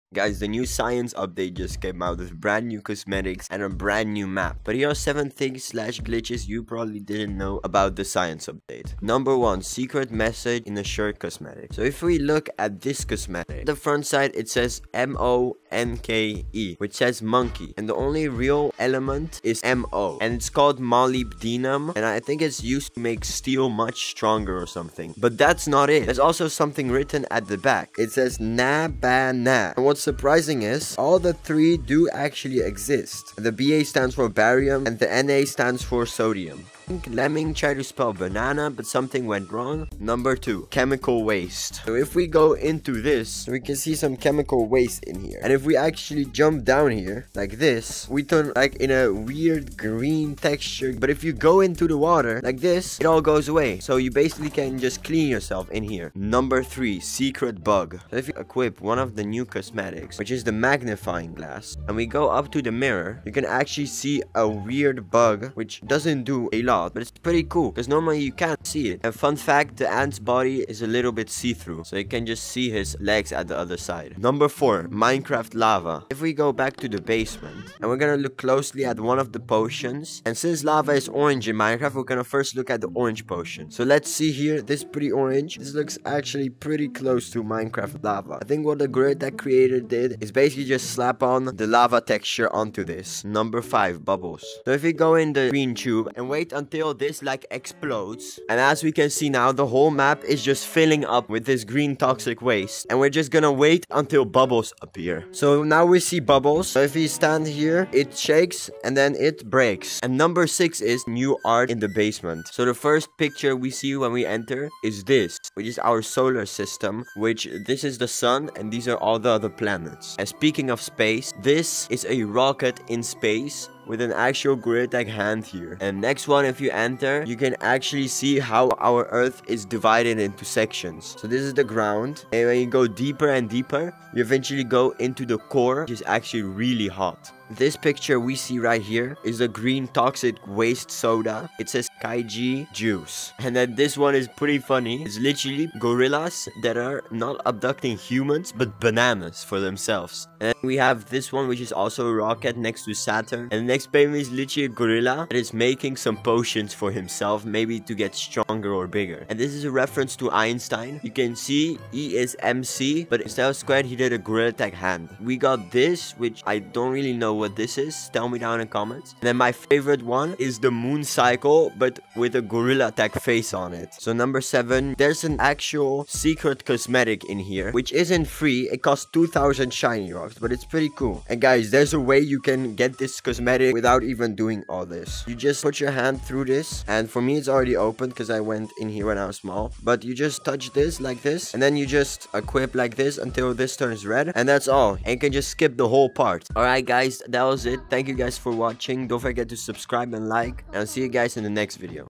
7 Things You Didn't Know About The SCIENCE UPDATE
So today we i will be showing you guys some glitches or things that you didnt know about the new science update!